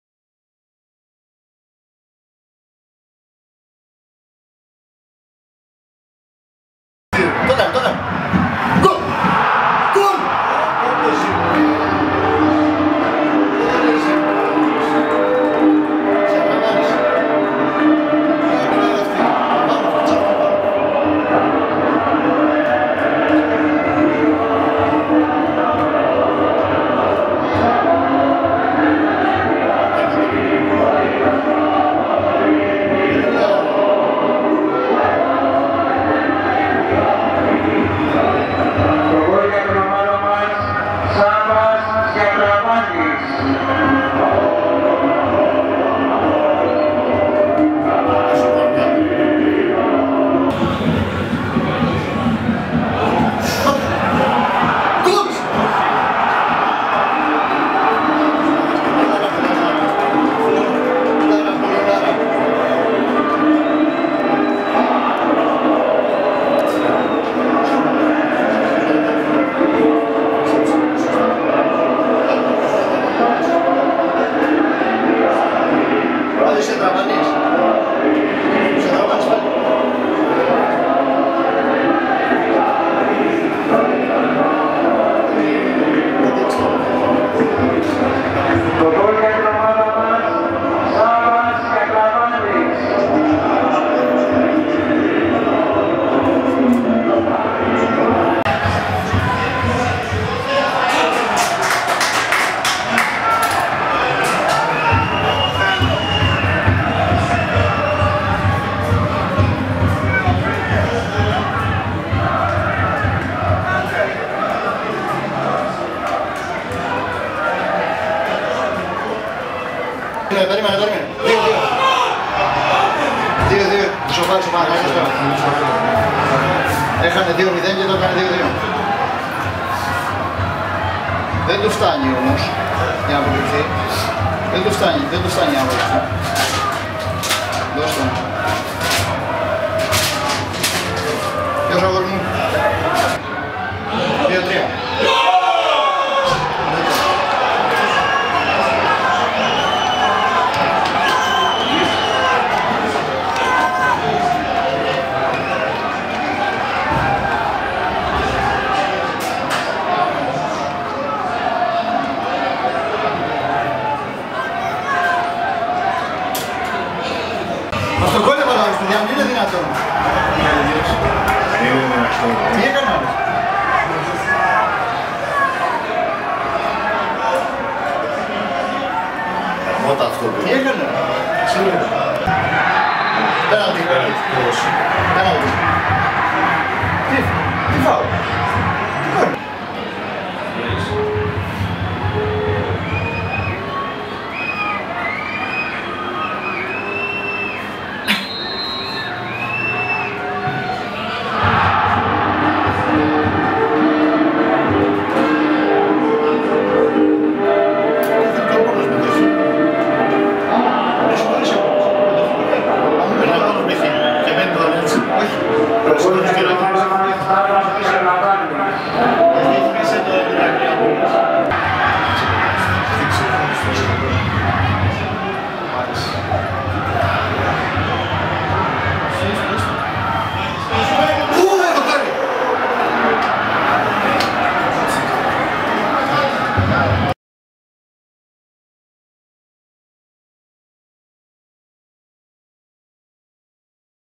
Απόλλων Σμύρνης - Παναθηναϊκός 3-4 video (15/12/2016)
Tα στιγμιότυπα του αγώνα της β' φάσης του Κυπέλλου Ελλάδας με φυσικό ήχο * Τα δικαιώματα ανήκουν στον OTE TV